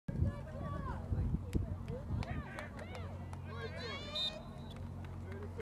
James carries but gets tackled quickly
Covenant Christian Cougars vs. CCS Lions